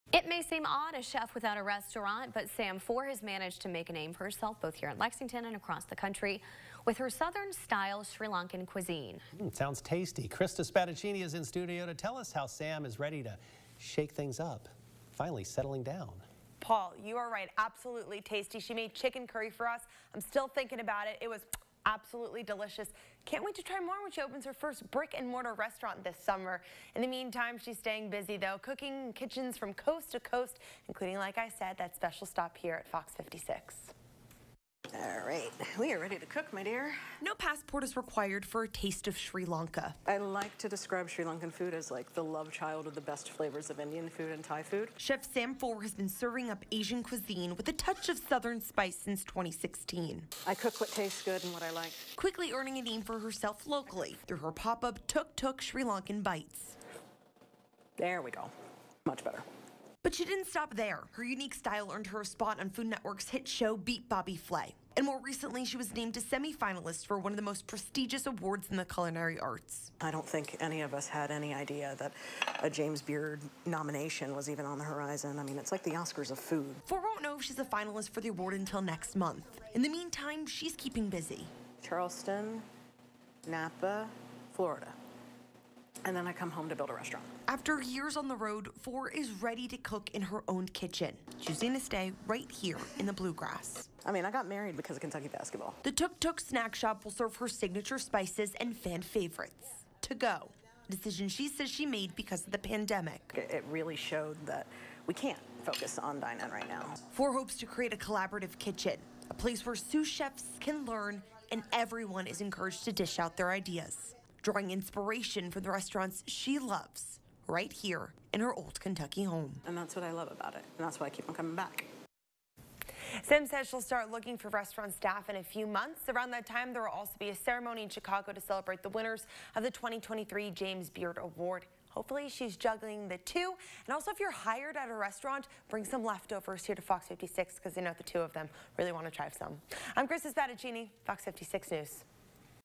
Big accomplishment for Kentucky chef
Sam Fore has managed to make a name for herself with her Southern-style Sri Lankan cuisine.